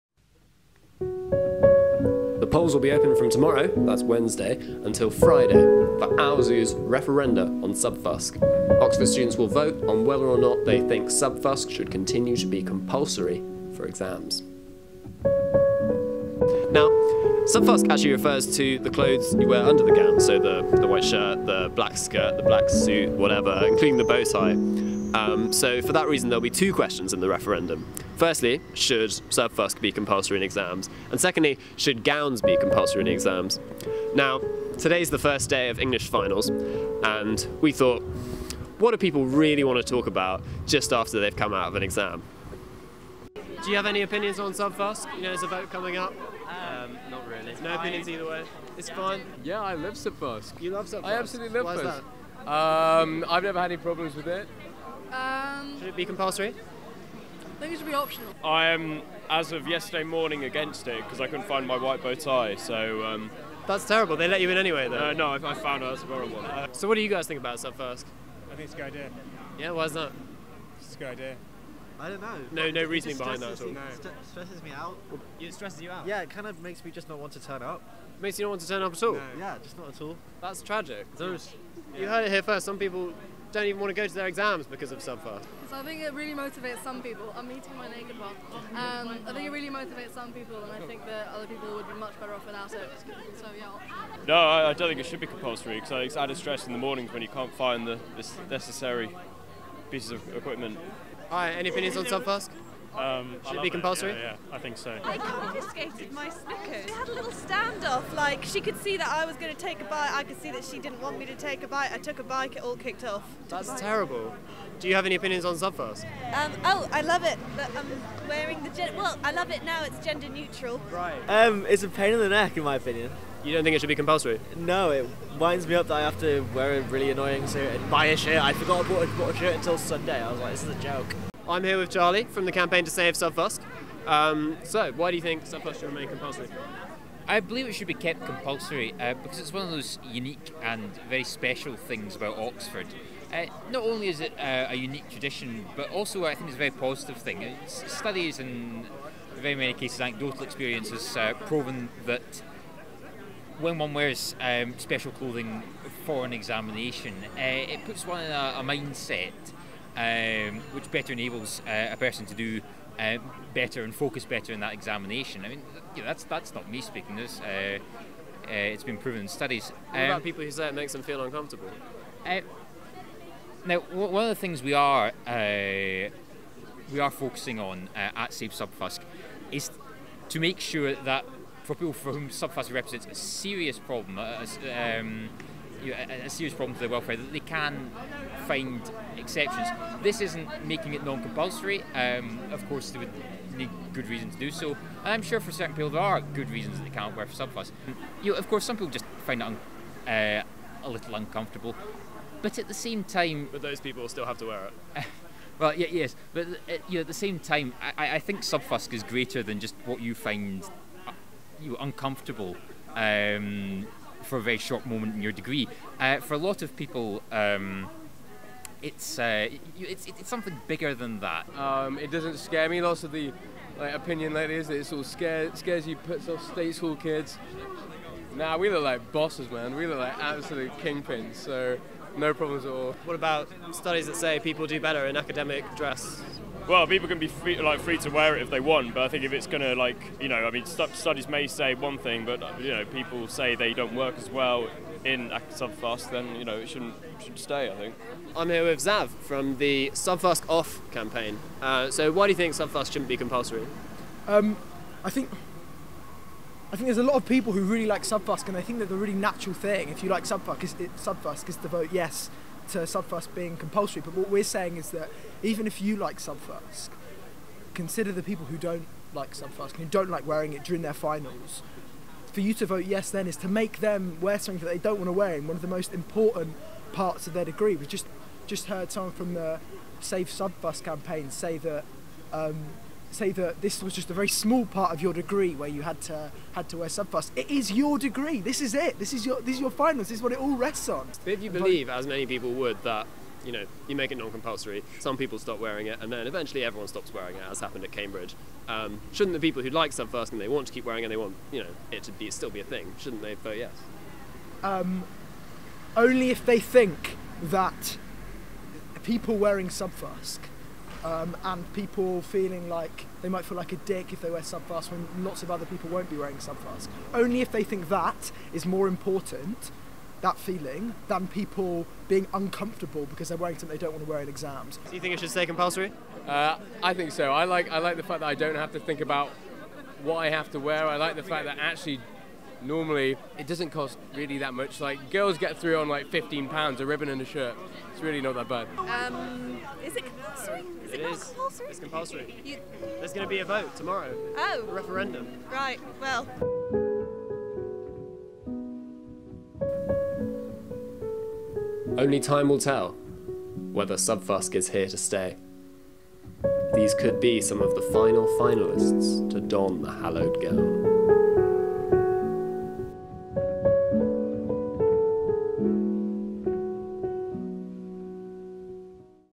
Should subfusc and gowns remain compulsory for Oxford exams?
Luke Barratt talks to English finalists and campaigners from either side of the argument about whether or not it should be compulsory to wear subfusc and gowns during exams.